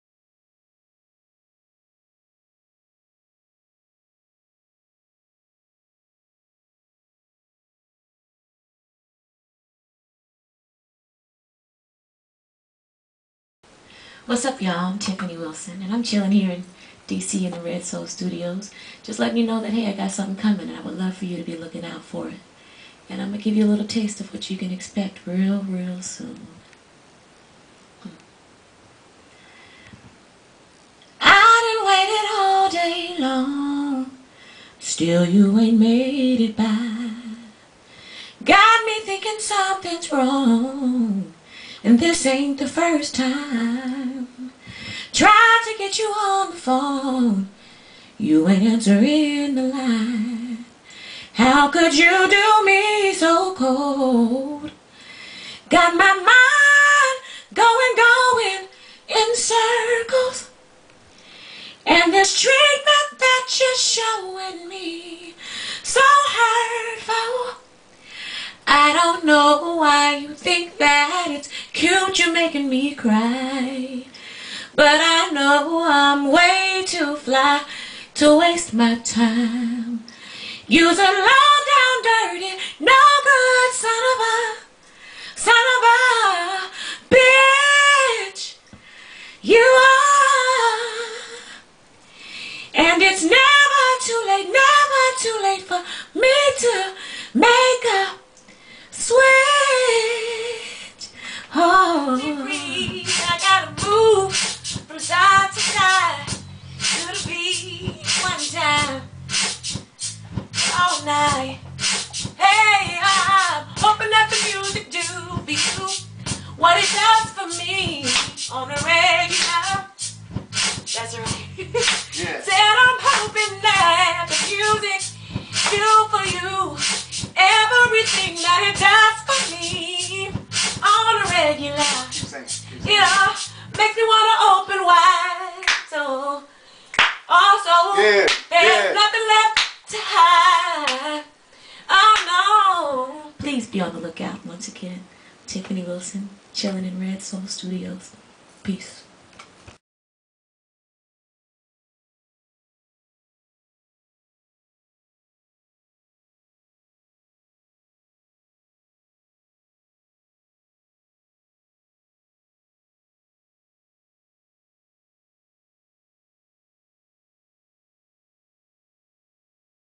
Tiffany Wilson live with DJ Conscience (R.E.D.SOUL Ent)
Tiffany Wilson Live at R.E.D.SOUL Studio with DJ CONSCIENCE. Giving you a taste of whats to come